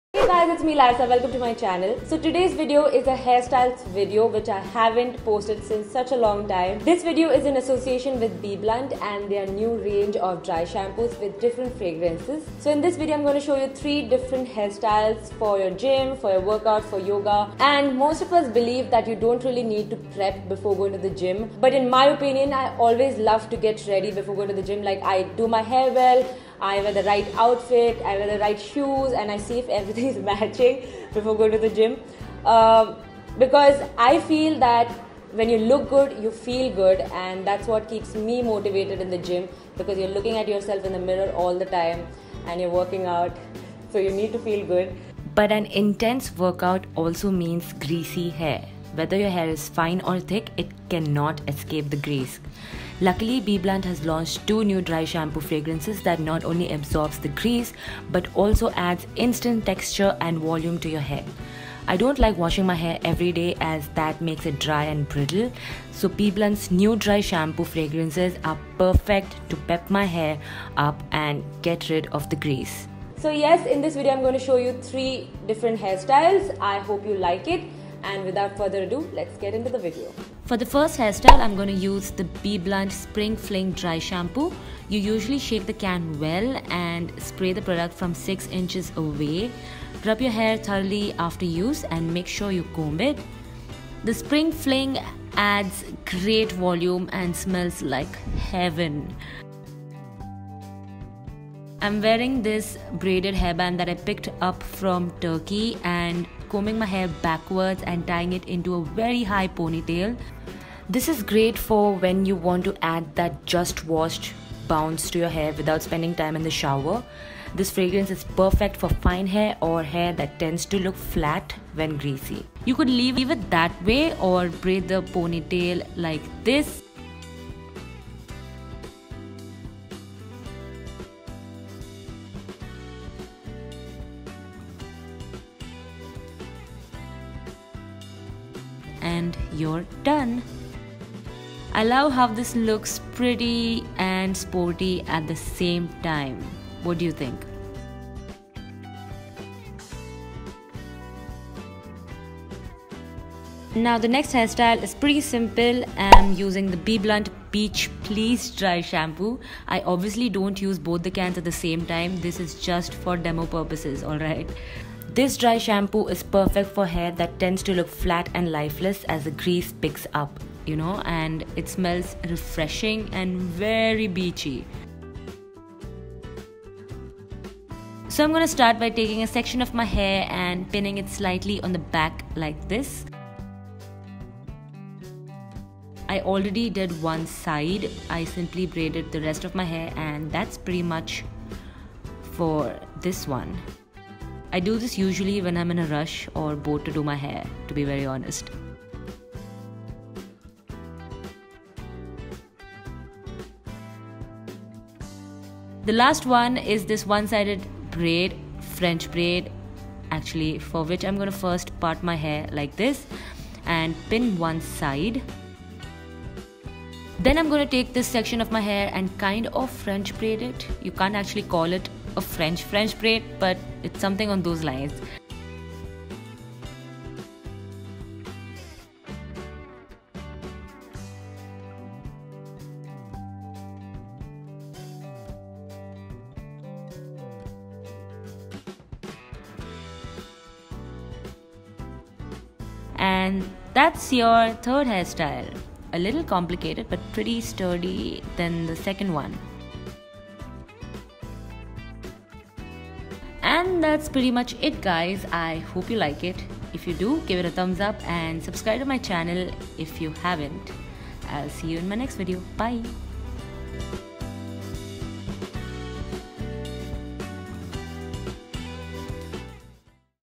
Braided Hairstyle for your Workout with Dry Shampoo | Larissa D'Sa
Larissa D'Sa uses the Back To Life Dry Shampoo to style a braided workout hairstyle.
She uses the Spring Fling Back To Life Dry Shampoo to add volume, and the Beach Please Back To Life Dry Shampoo to add texture.

Know more about BBLUNT's range of products made specially for Indian hair, weather and water, & our salons here: